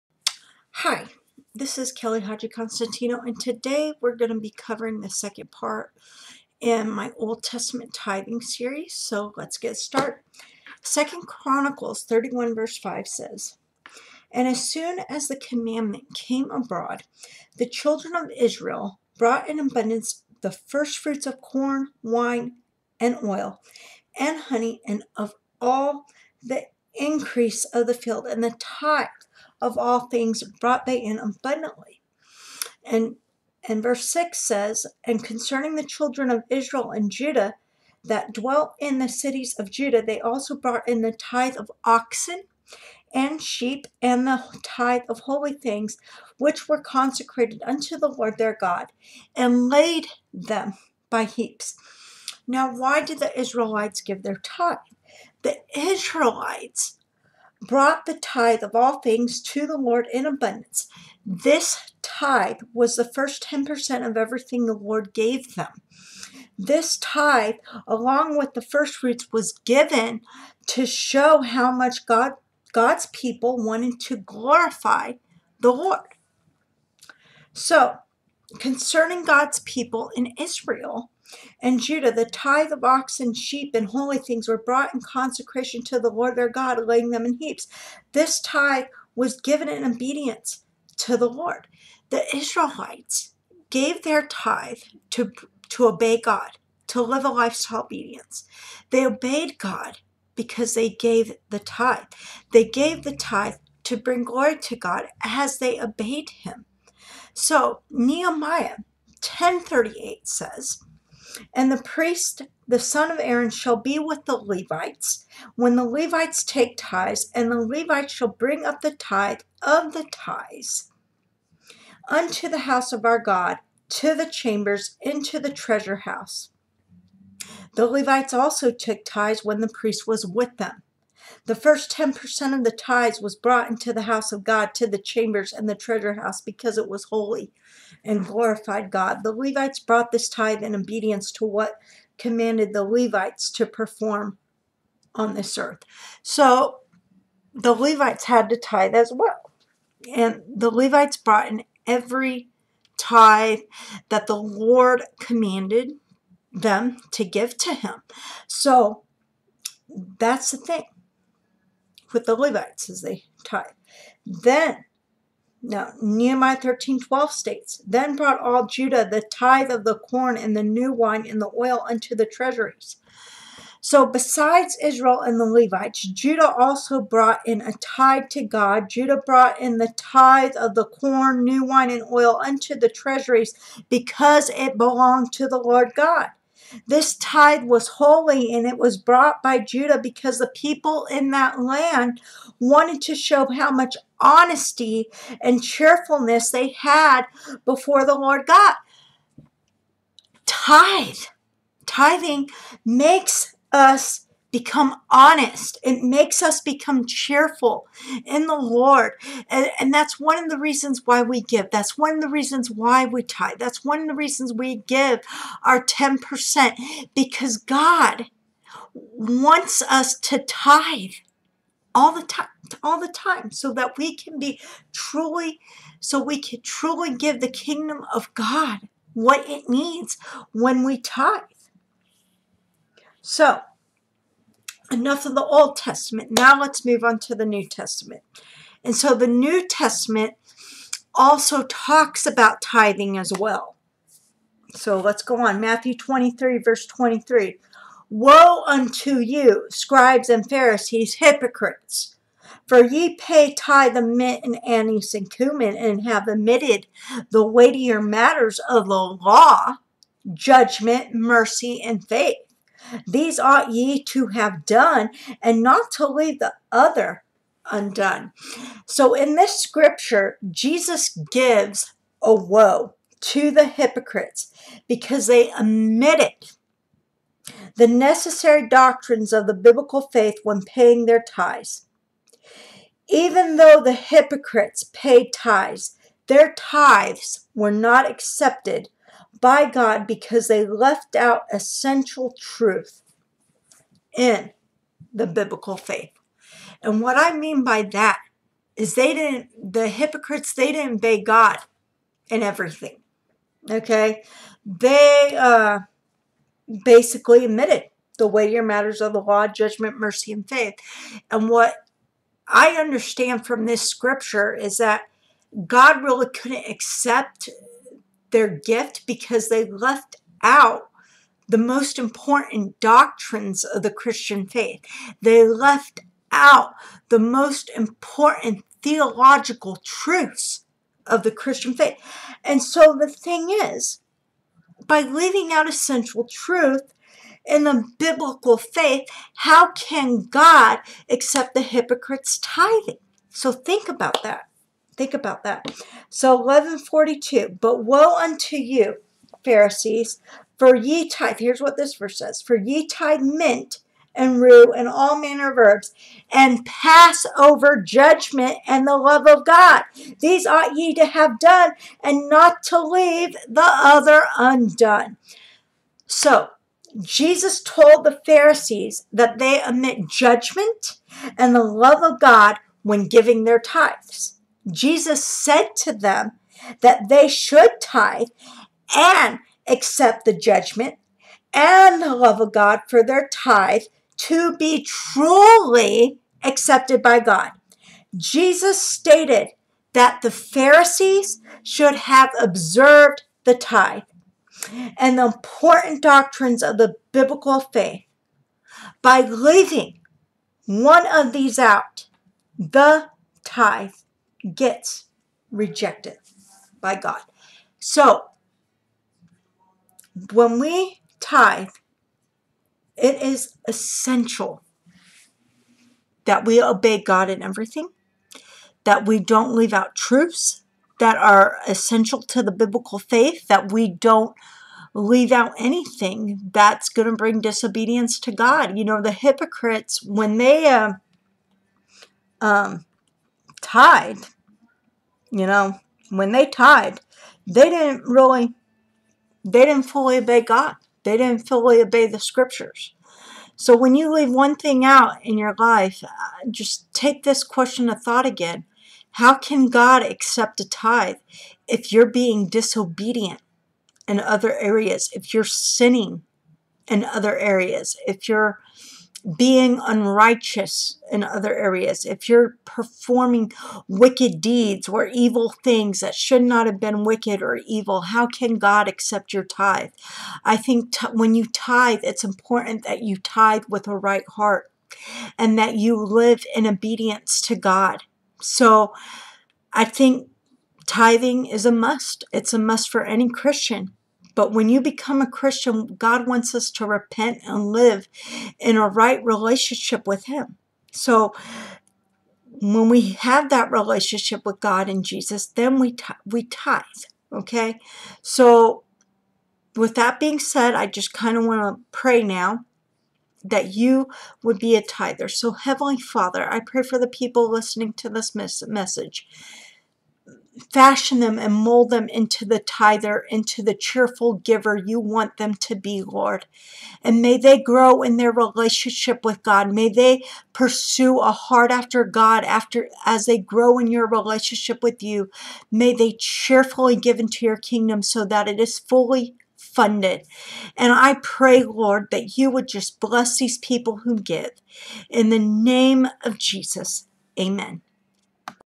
Tithing Essentials Part 2!!!
In this video, the second part of my tithing series is discussed. We cover tithing in both the Old Testament and New Testament. By learning about tithing, you can become a cheerful giver in the Kingdom of God.